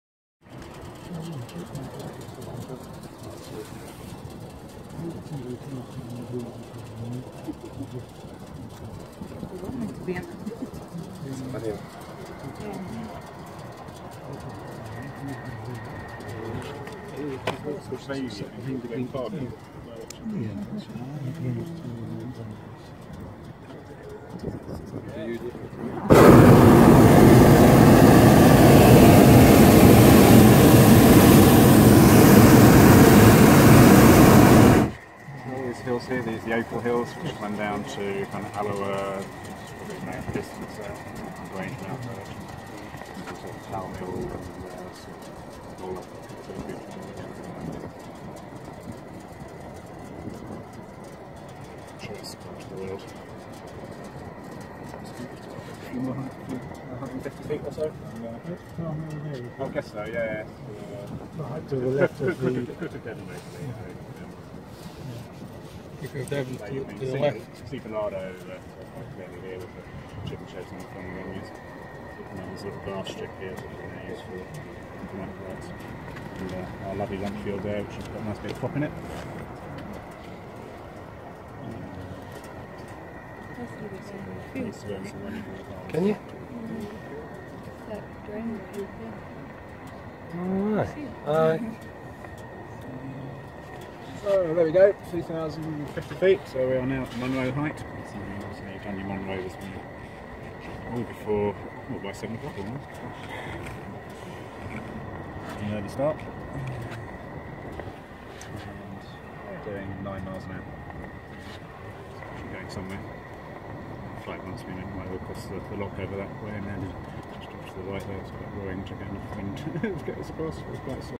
Hot Air Balloon flight over Fife & Kinross
A lovely early morning flight in a hot air balloon with me and Nicole. Start in Kinross then float over Fife. The weather isn't normally this nice so we were lucky.